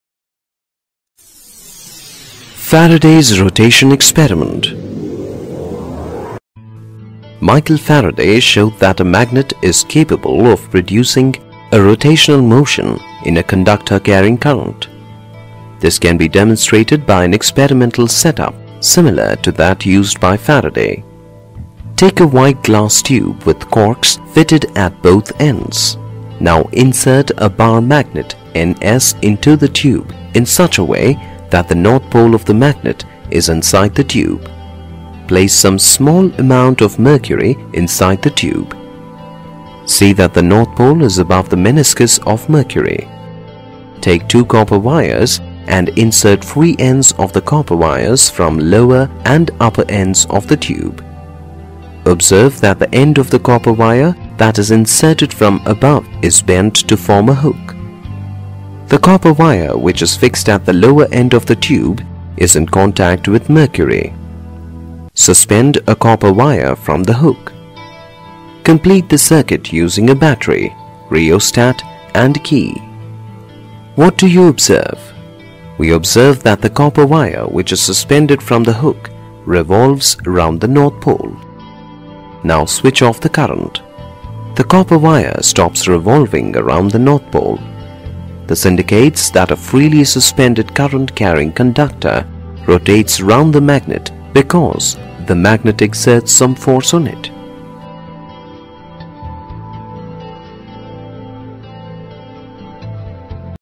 Faraday's Rotation Experiment
Faraday's Law

The induced emf is equal to the negative rate of change of magnetic flux through the coil.